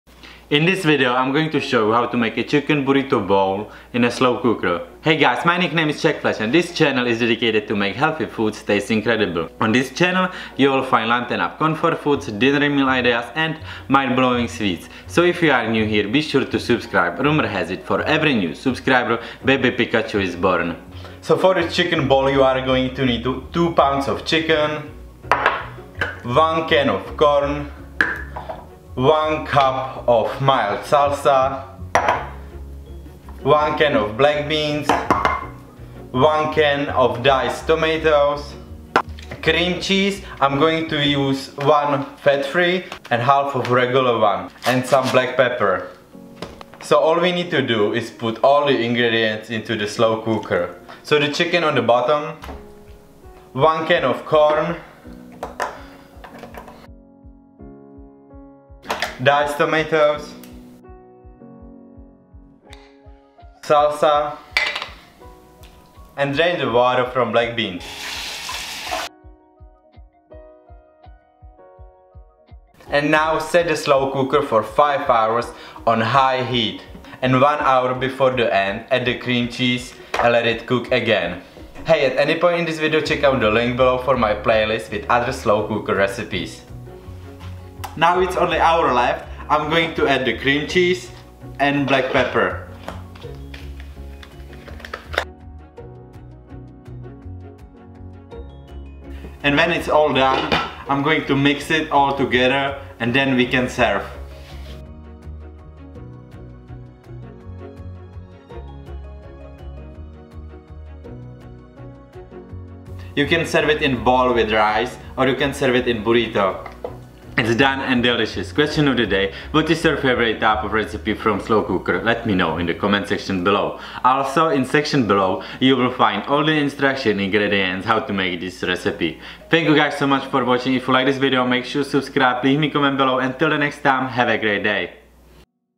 Slow Cooker Chicken Burrito Bowl
Healthy Chicken Burrito Bowl:

2 pounds of chicken breast
1 can of corn
1 can of diced tomatoes
1 can of black beans
1 cup of medium salsa
1 serv. of fat free cream cheese
1/2 serv. of cream cheese
pepper

neděle 5. února 2017
14:49

My gear -
My camera
My Microphone
My second camera/phone
Mixer I usually use
Favorite crock pot